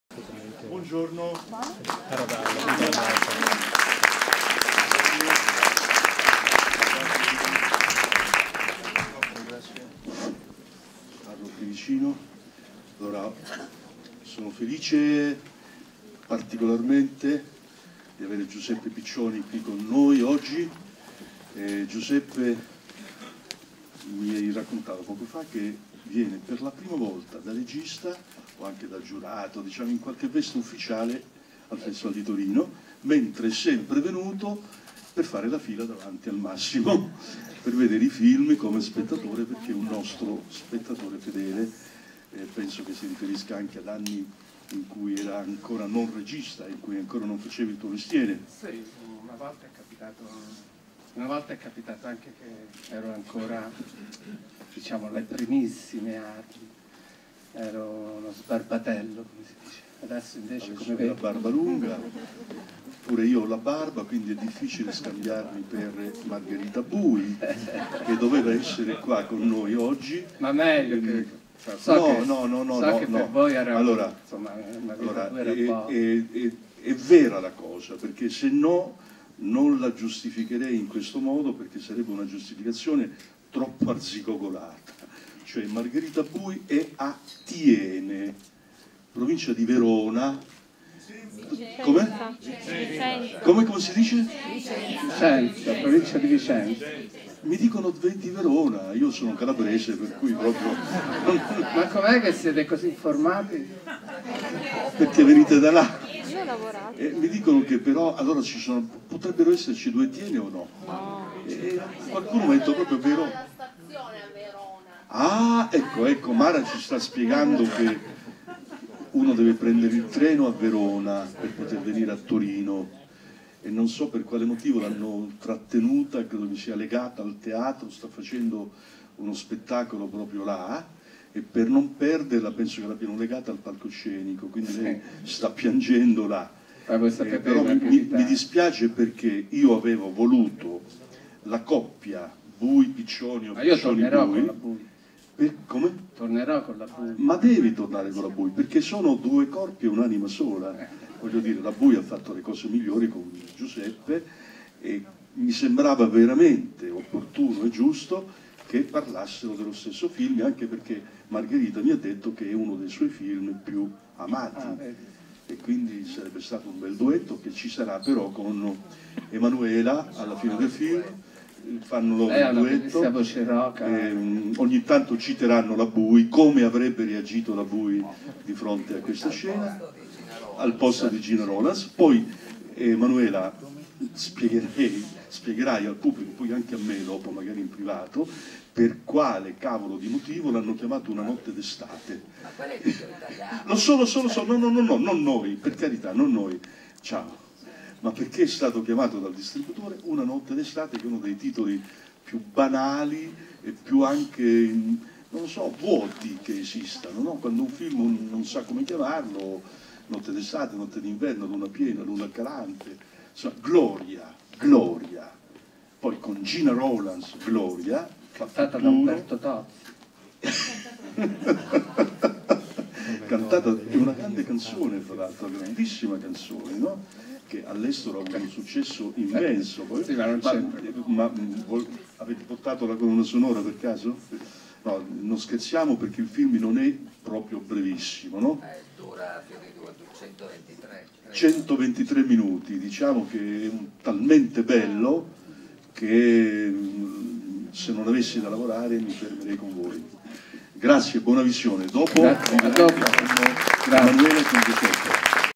GLORIA presentazione 30TFF
Gianni Amelio e Giuseppe Piccioni presentano "Gloria" di John Cassavetes TFF30